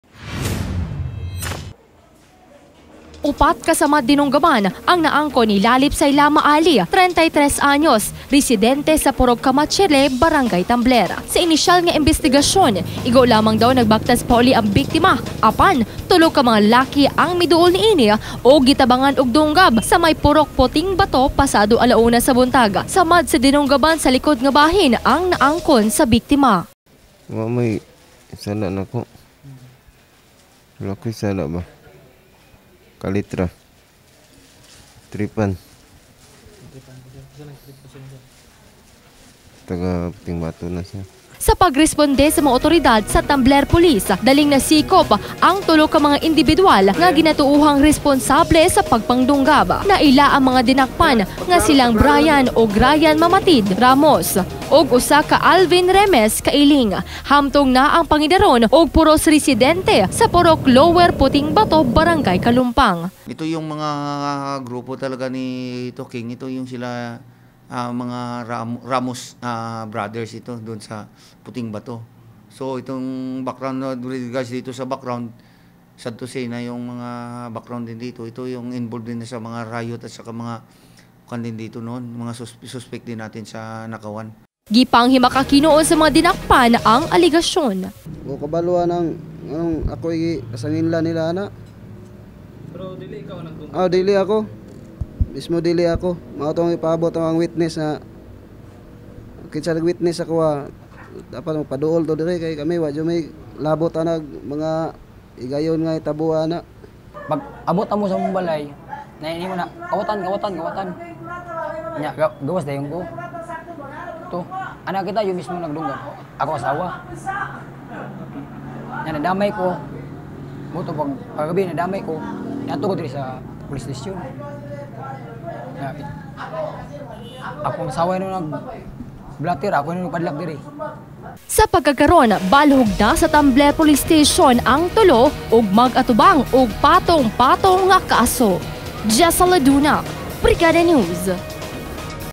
BNTV - Gensan - RONDA BRIGADA - NAGBAKTAS LANG PAULI - JULY 23, 2015
NAGBAKTAS LANG PAULI GIDUNGGAB, ANGOL! - Report ni: Jessa Leduna